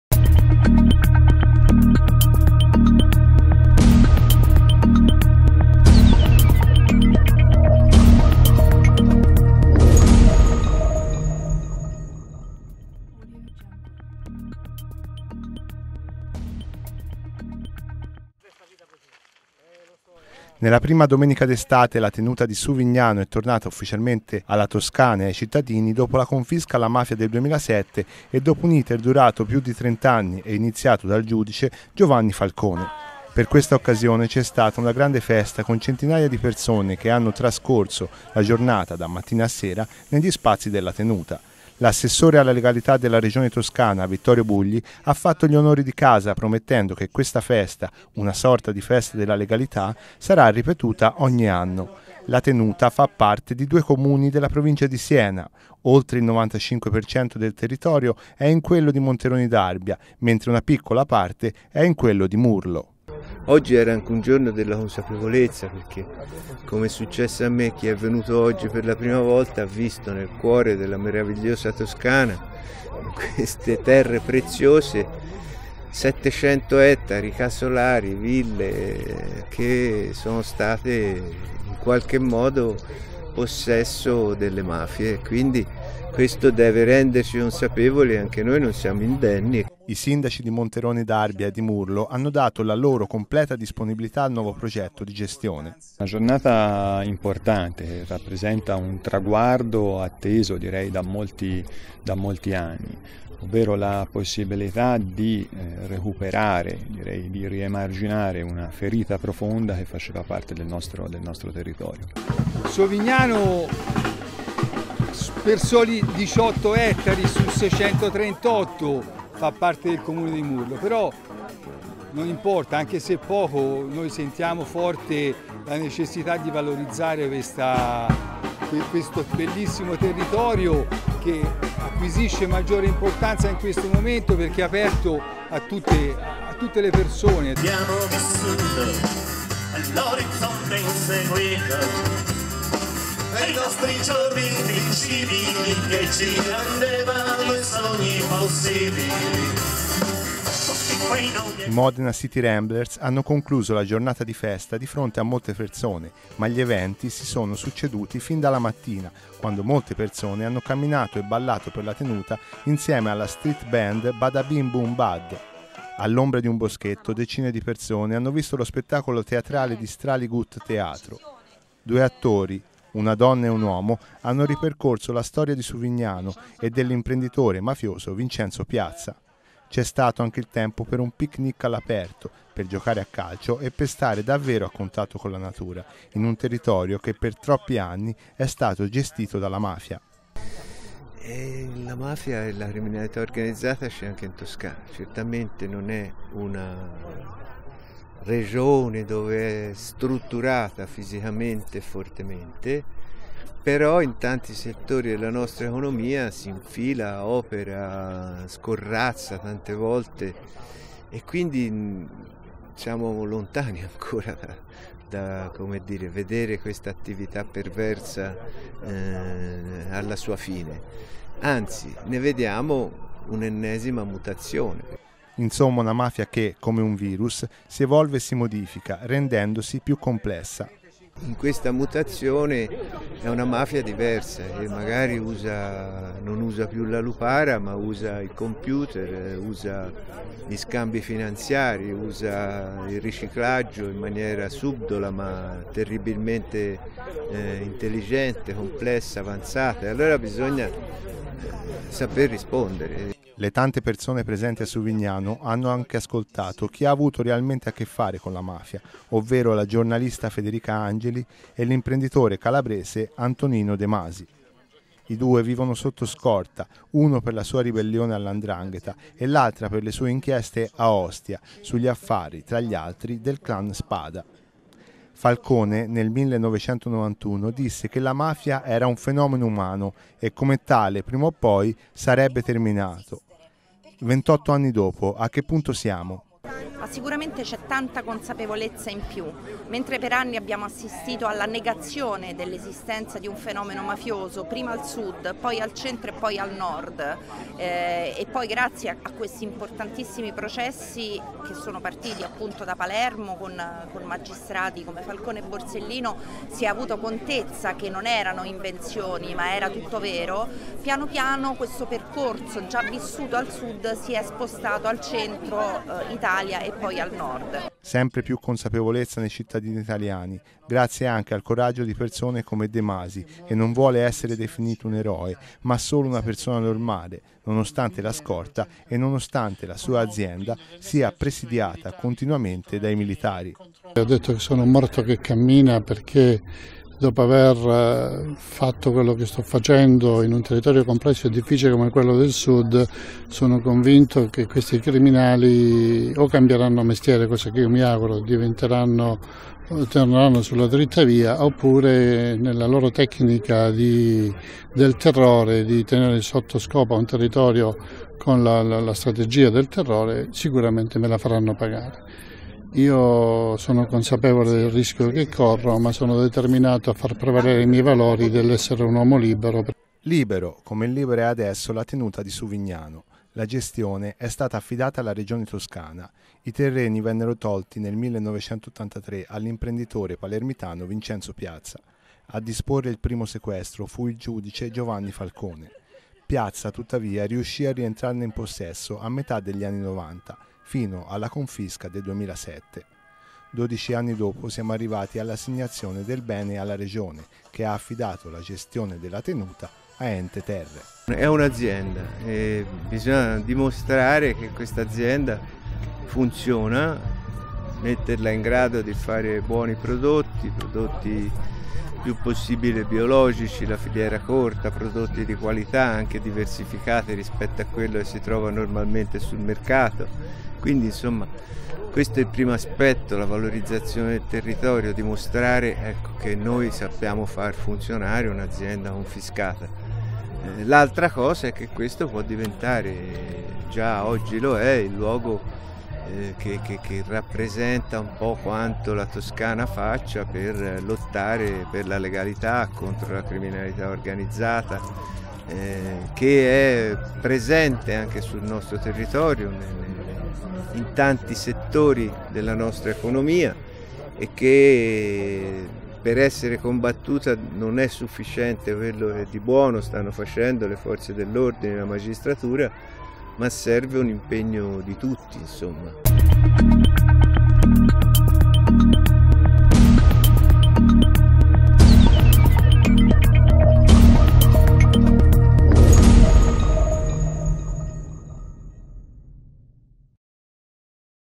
Suvignano, un giorno di festa
Cancelli aperti per celebrare una vittoria della legalità e della democrazia sui poteri criminali. La notizia